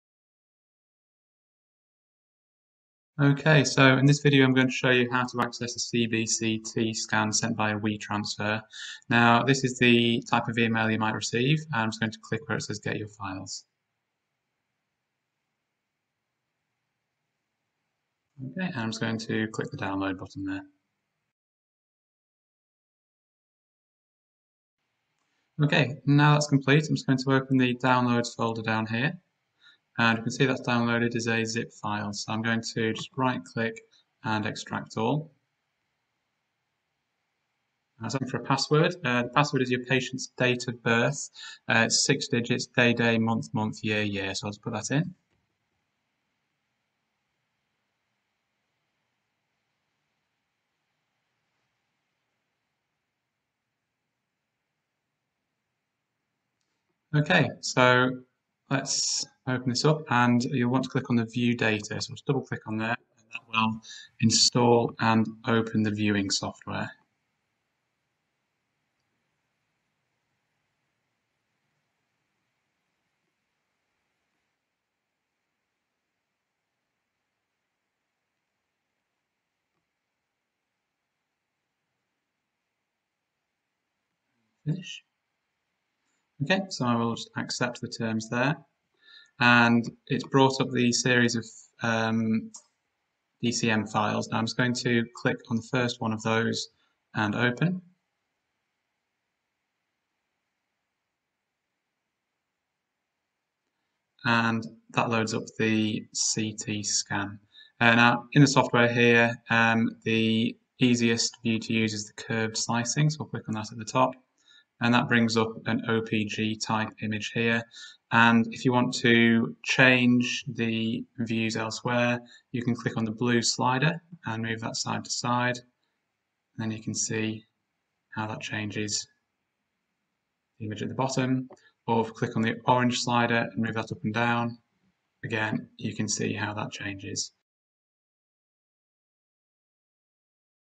How to download a CBCT scan sent via WeTransfer
This video shows you how to download and access a CBCT scan which we have sent to you via WeTransfer. The download includes free Carestream Viewing software